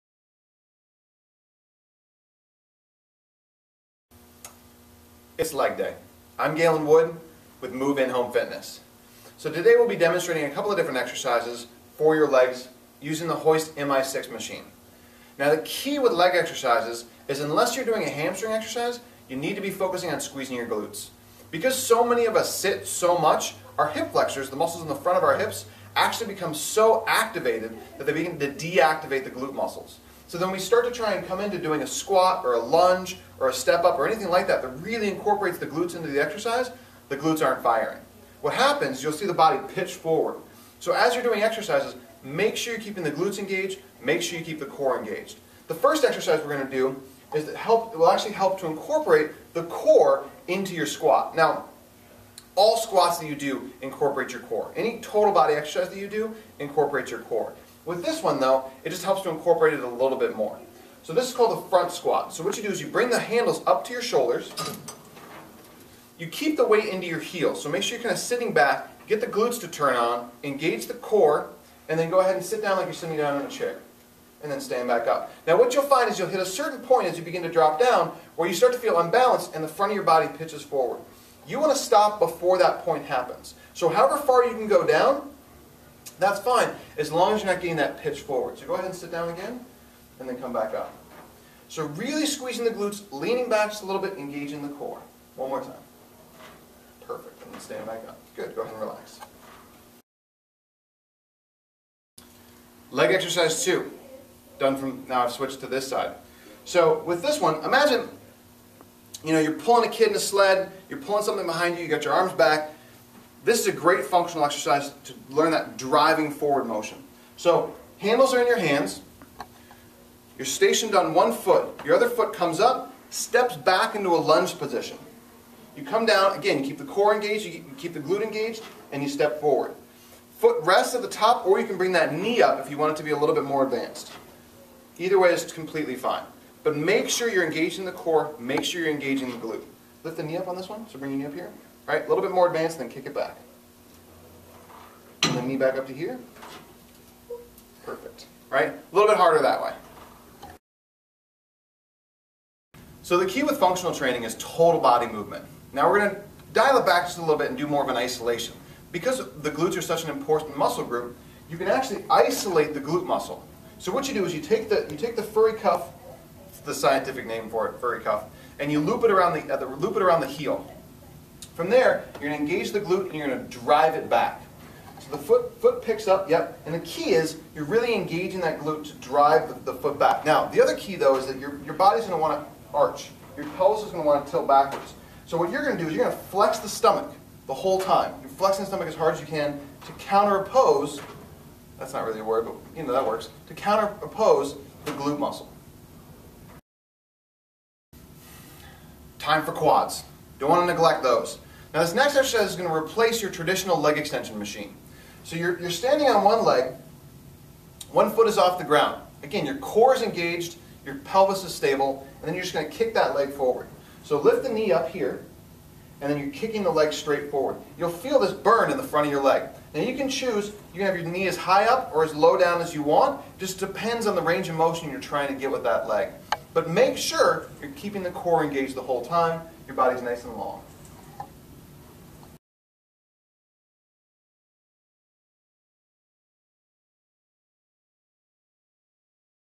Hoist Mi6 - All Legs
Leg exercises on the Hoist Mi6 Functional Trainer at the 2nd Wind Richfield, MN store